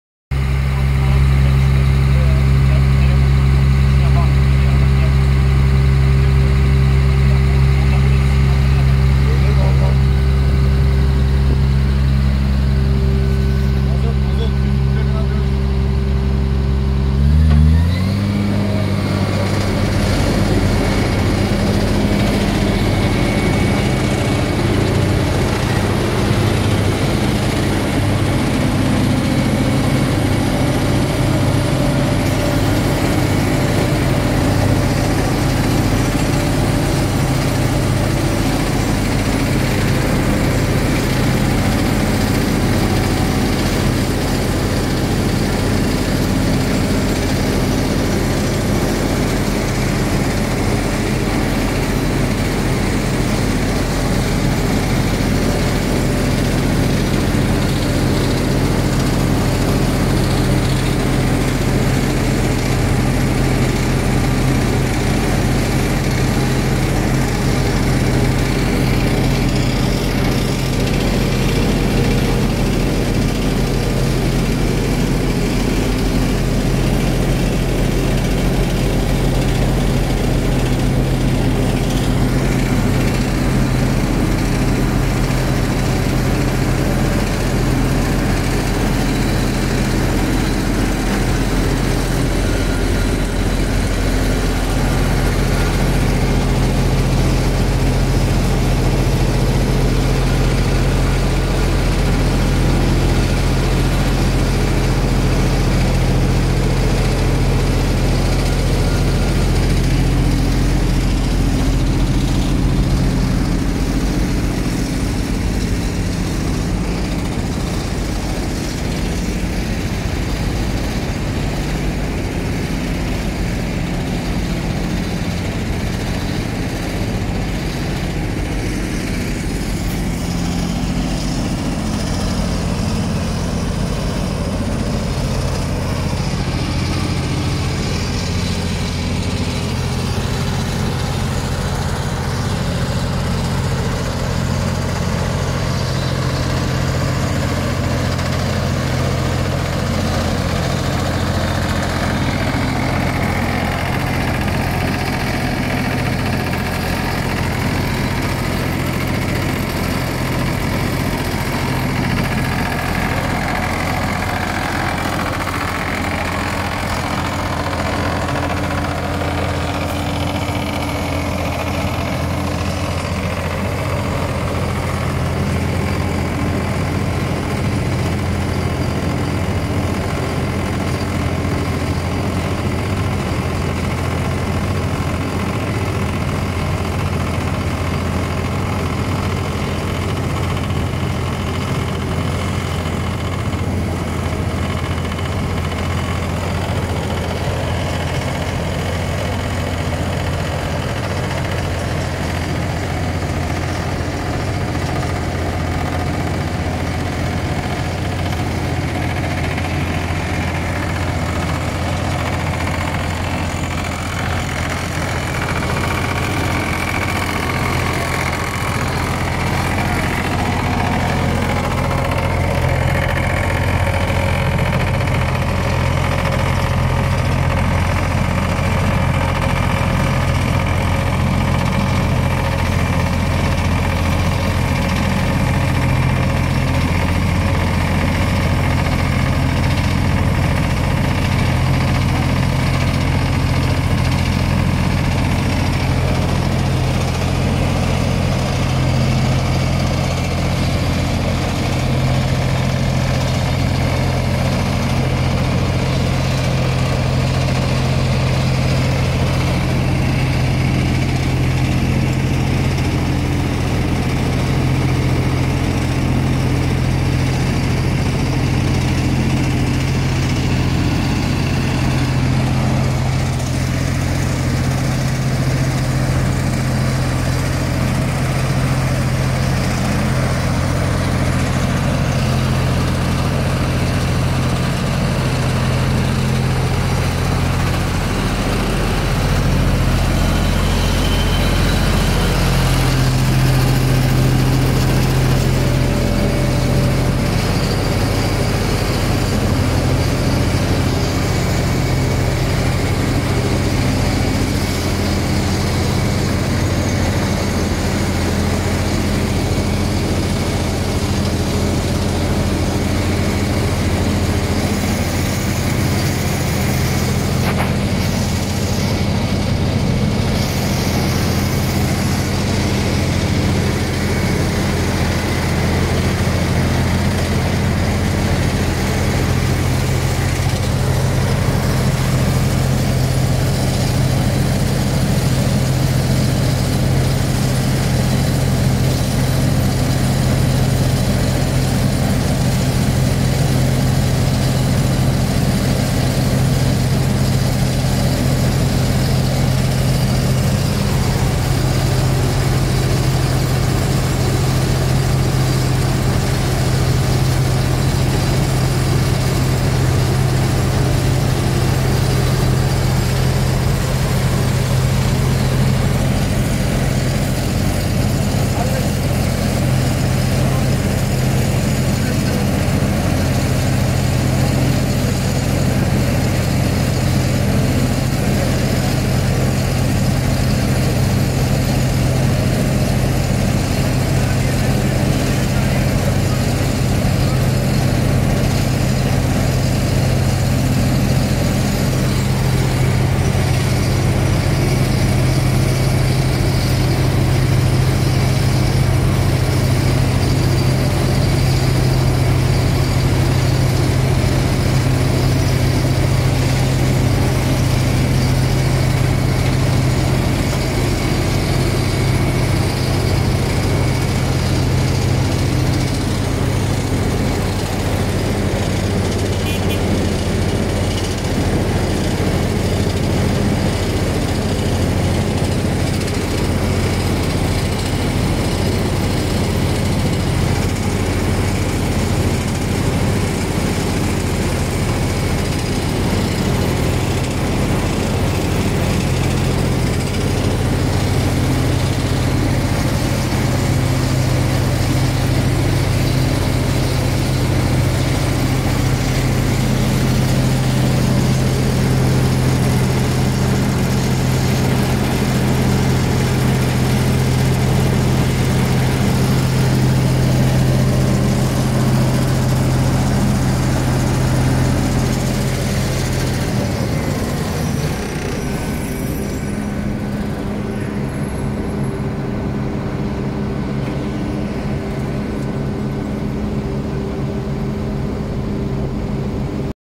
Hajiawa Dual Road paving of wearing Layer 4 cm thickness after compaction, 12 m width...
Hajiawa Dual Road paving of wearing Layer 4 cm thickness after compaction, 12 m road width, Asphalt paver can open more than 12 m